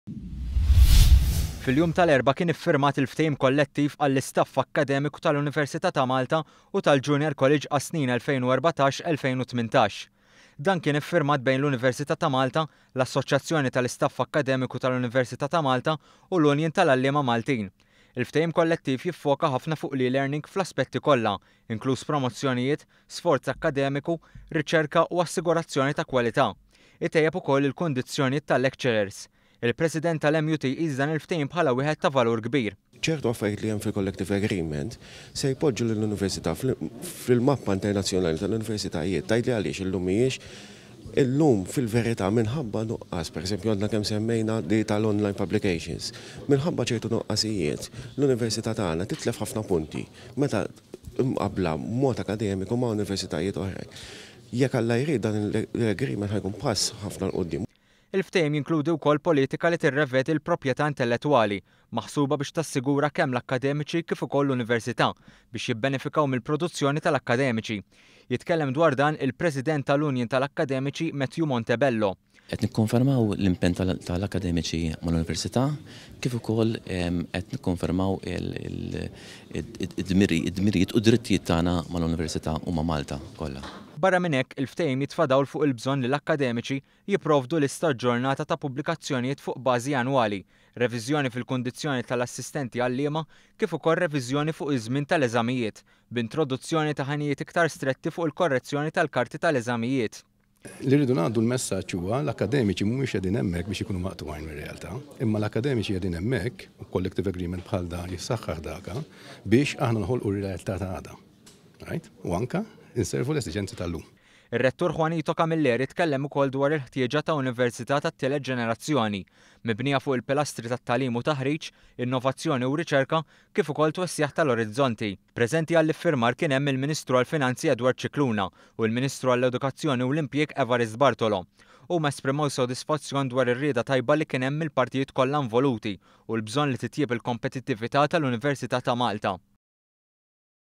Iffirmat ftehim kollettiv bejn l-Universita’, l-MUT u l-UMASA.
Ftehim kollettiv li se jkun qed jiffoka fuq l-aspetti kollha li għandhom x’jaqsmu mal-eLearning.